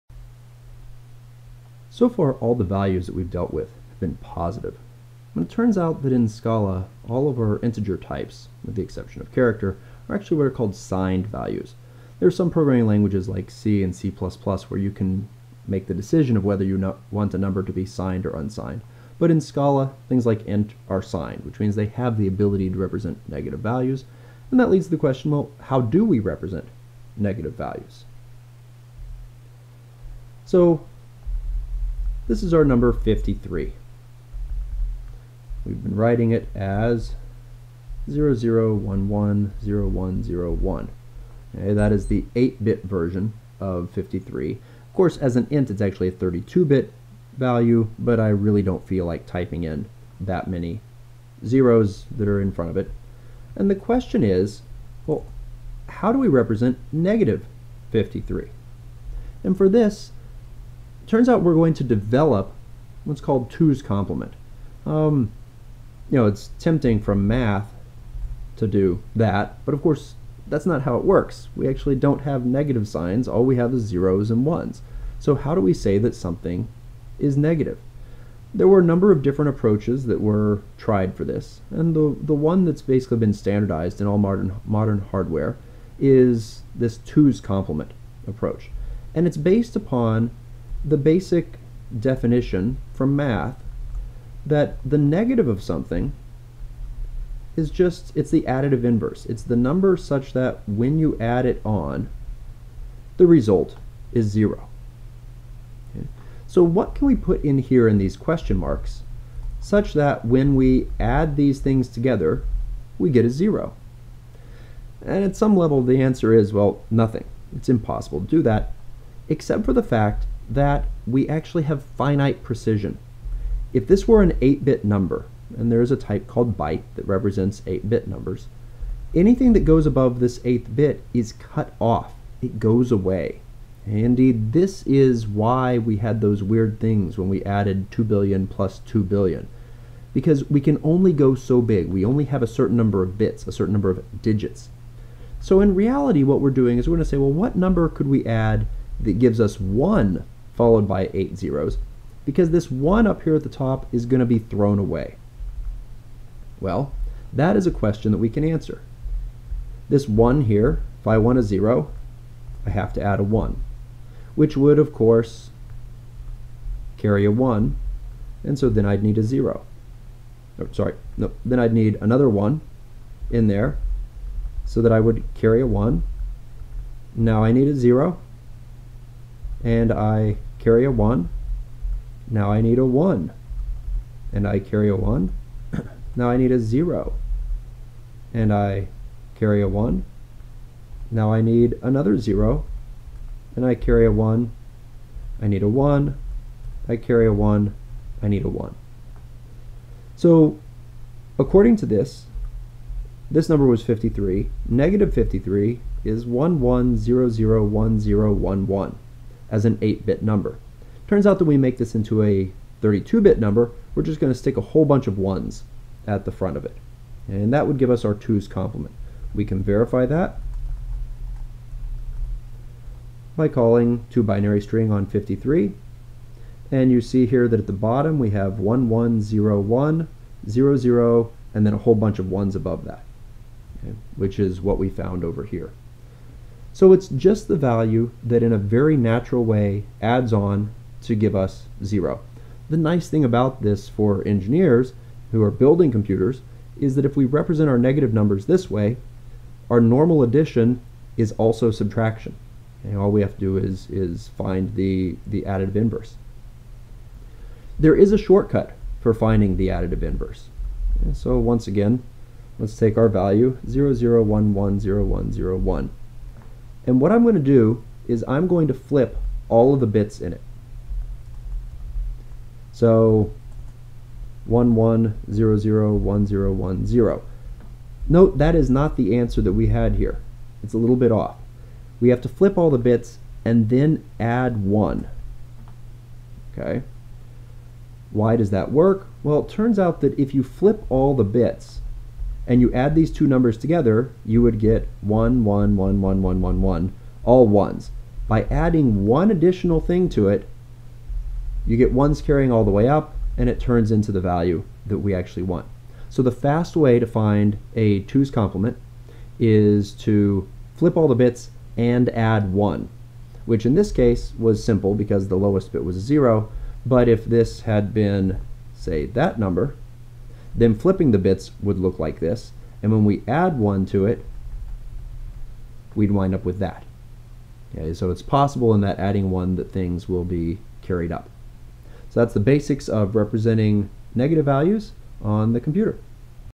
Signed Numbers and Negative Values (Scala Basics)
This video shows you how to get a two's complement negative number for a binary value.